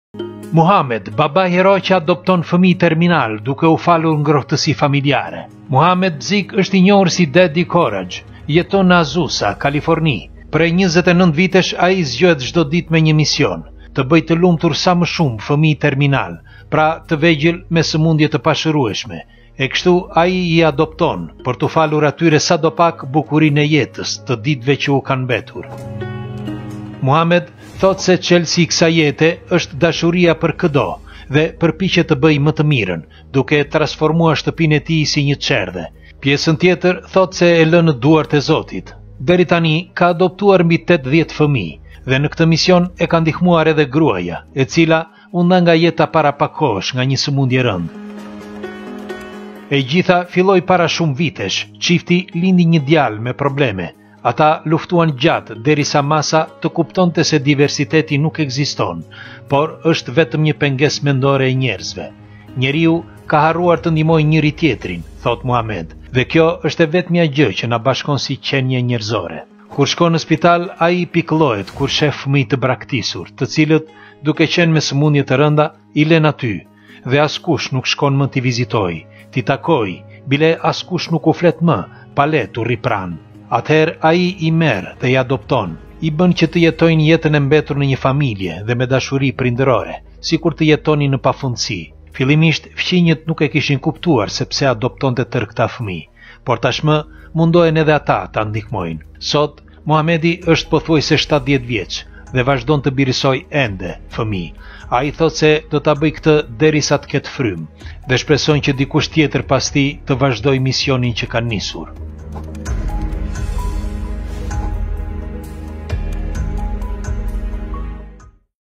Mohamed BABAI HERO QE ADOPTON FEMIJE TERMINAL DUKE U FALUR NGROHTESI FAMILJARE😘
Te ndihmosh dike tjeter qe ka nevoje dhe qe nuk e kupton eshte njesoje sikur dikush me i zhvilluar se ti te te ndihmoj ty nese ke nevoje